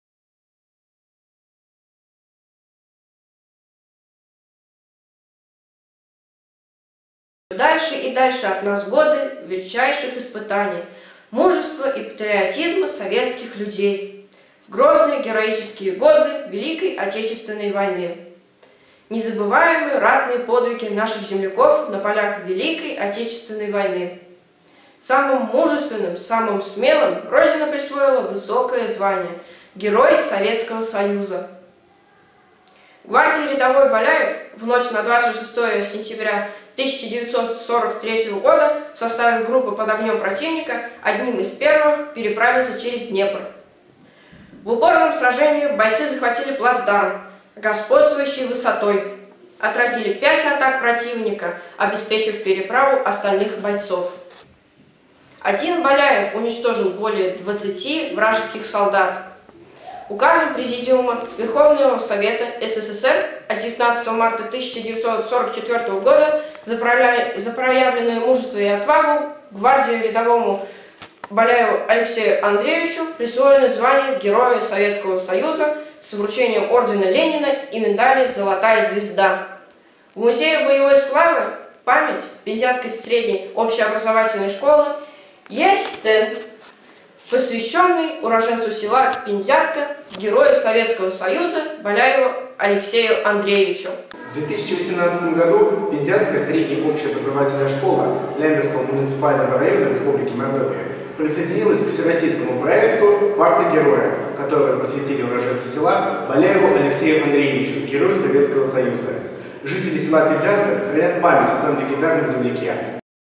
Герой советского союза Баляев Асым Айзятуллович
Герой советского союза, участник Великих военных действий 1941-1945 гг. Уроженец Республики Мордовия. Награжден боевыми медалями, орденами и почетными званиями за доблесть и отвагу во времена Великой Отечественной войны.